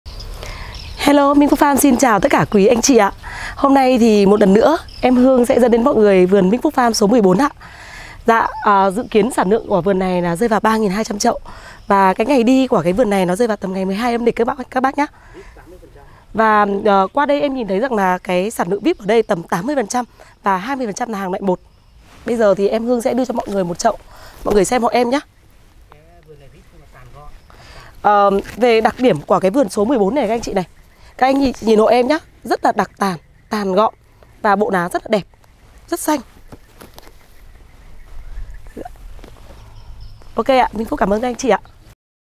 Vườn 14 Cúc Mâm Xôi 2024 Minh Phúc Farm. Sỉ sll cúc 0982051290- 0942138999
- Sản lượng khoảng 3200 chậu
- Ngày đi hàng rơi vào ngày 12 âm lịch
- Tỷ lệ của vườn số 14: 80% là hàng loại vip, 20% là hàng loại1
- Đặc tàn, tàn gọn bộ lá rất đẹp, lá rất xanh.